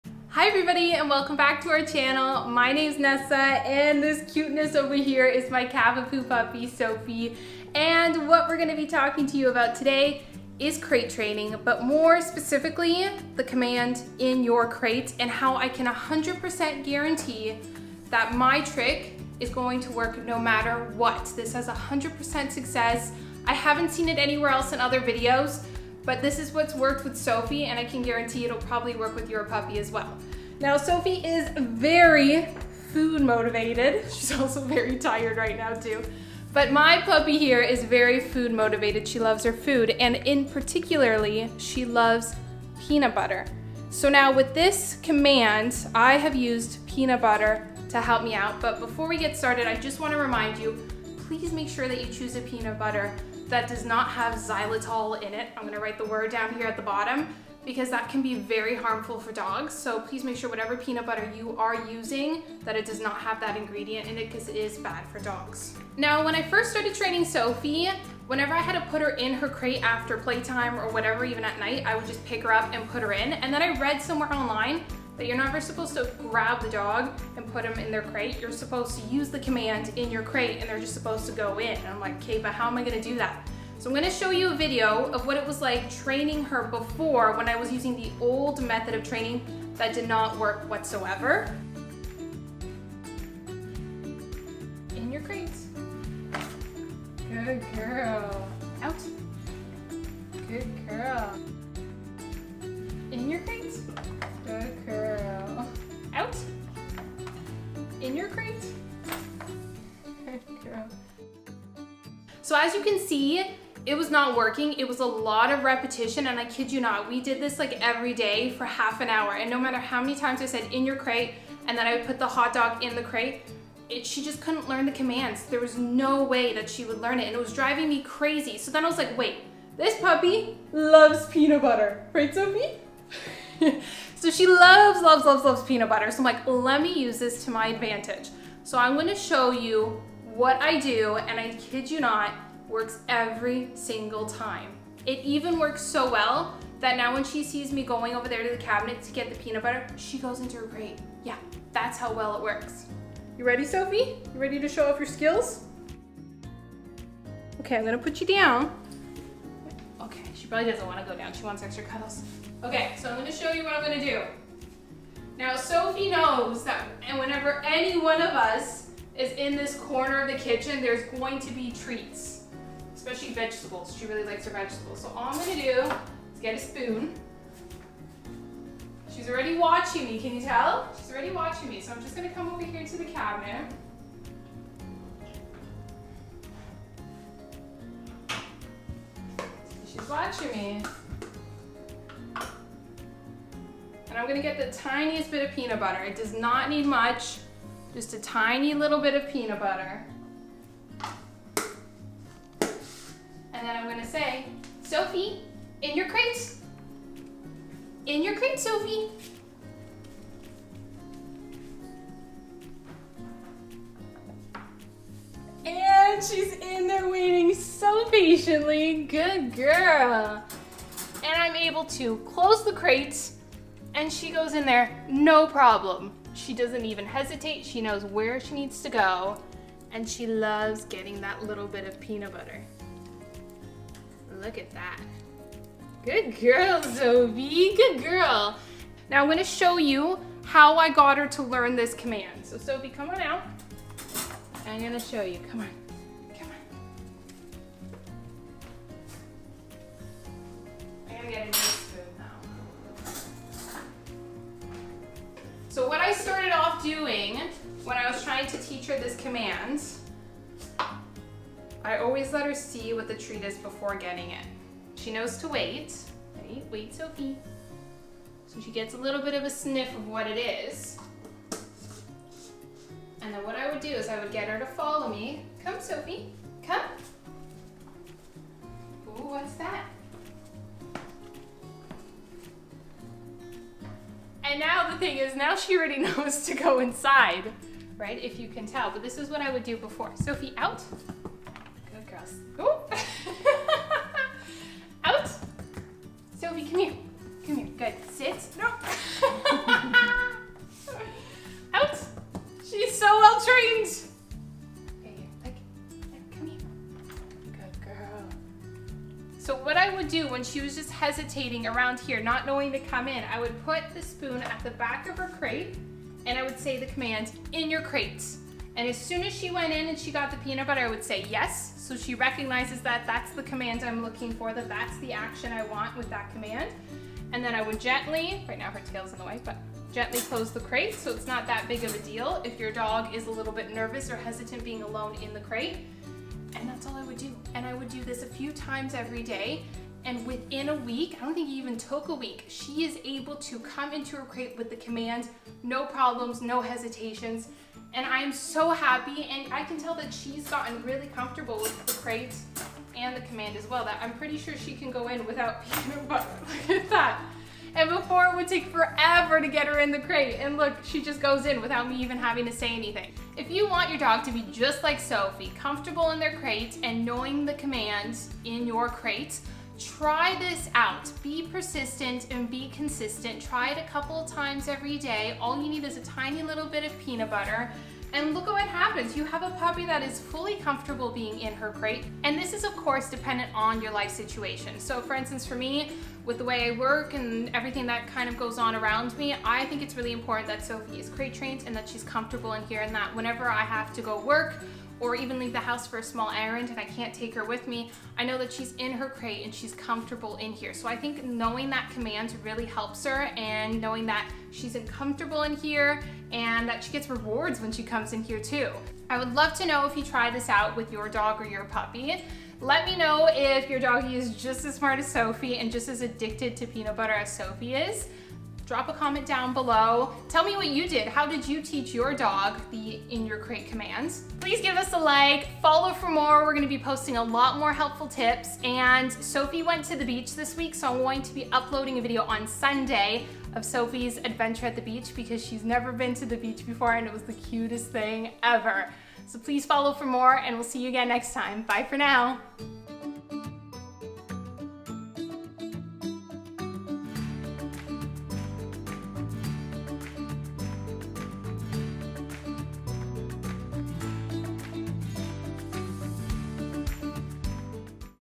Teach Your Dog the 'In Your Crate' Command in Less Than One Week
Have you been struggling to teach your dog or puppy the "in your crate" command like I was? I'm going to share with you the trick I use to get Sophie to go happily into her crate! I think this is one of my favourite things that I've managed to teach Sophie.